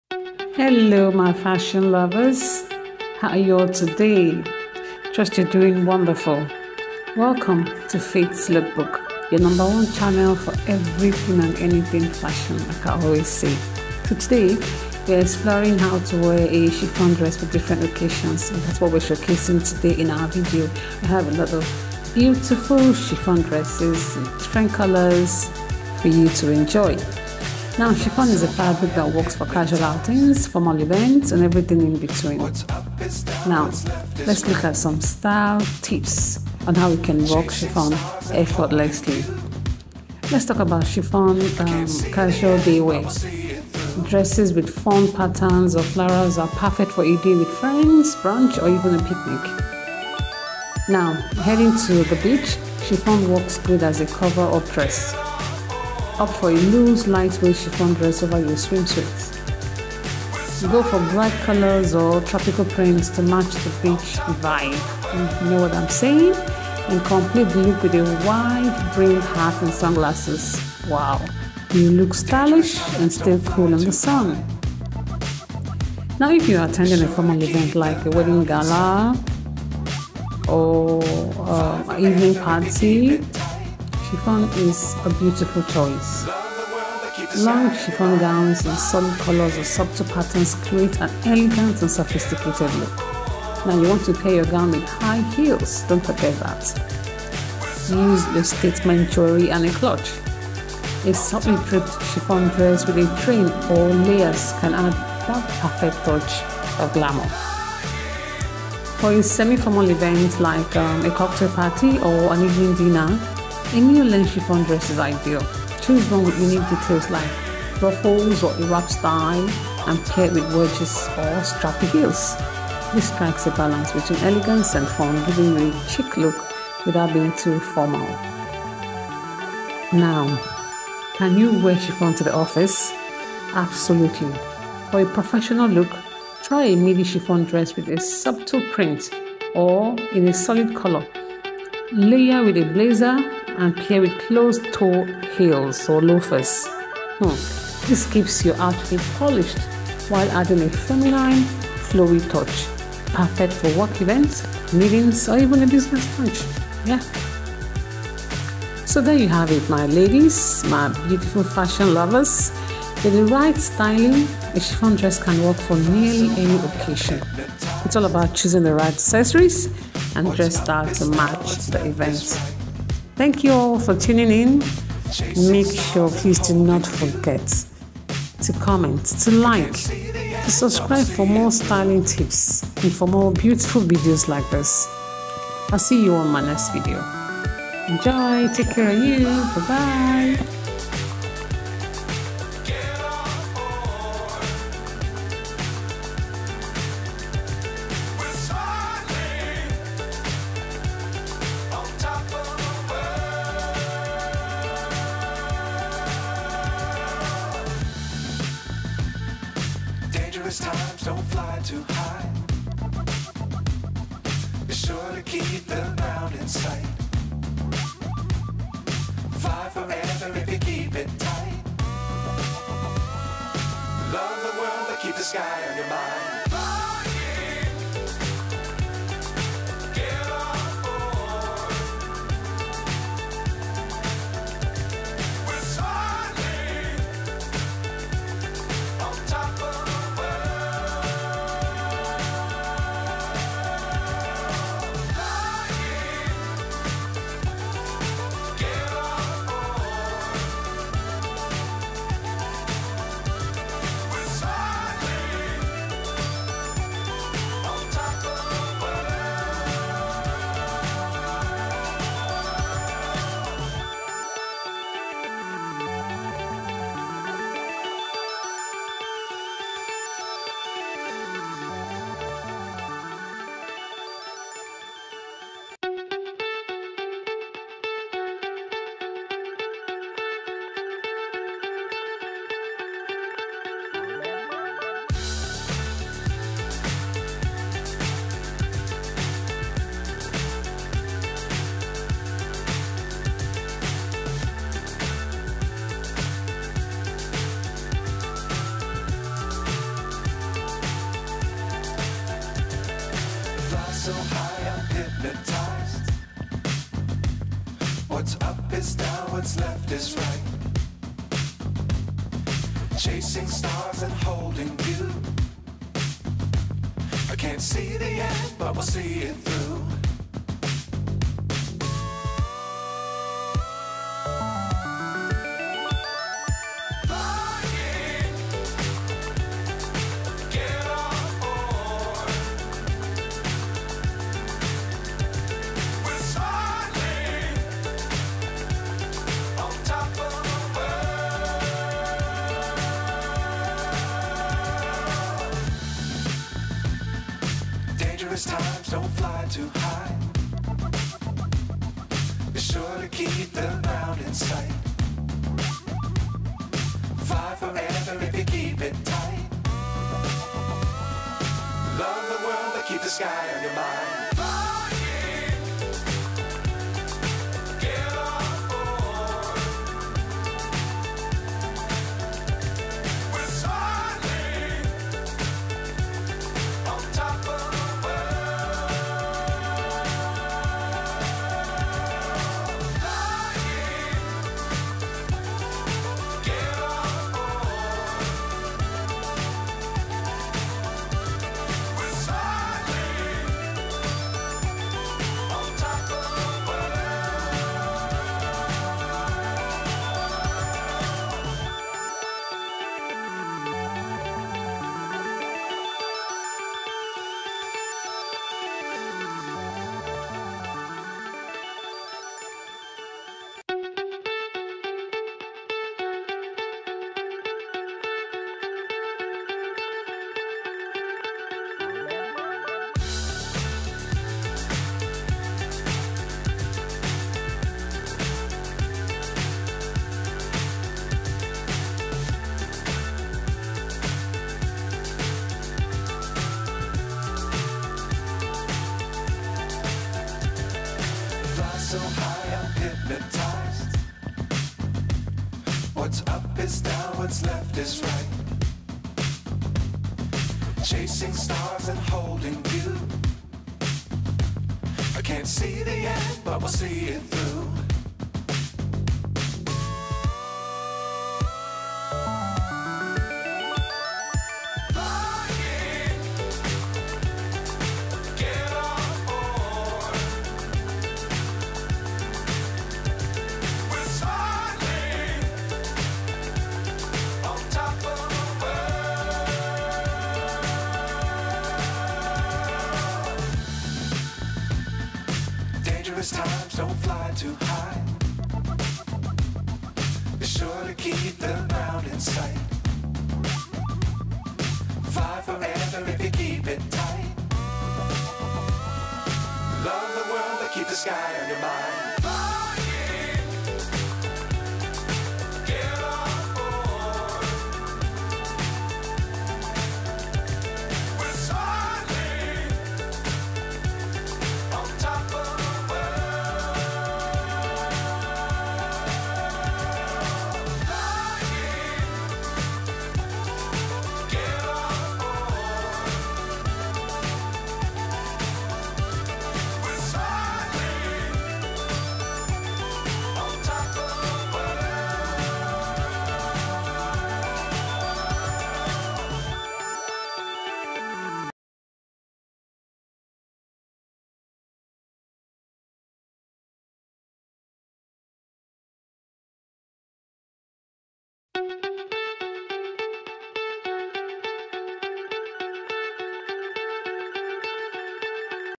Turn Heads With These Fabulous CHIFFON Dresses🌹 Latest Chiffon Dress For Fashionable Ladies#chiffon
Welcome to faith'sLookBook...

 In this video you will find Gorgeous chiffon Dresses created just for you... In vibrant beautiful colors and elegant styles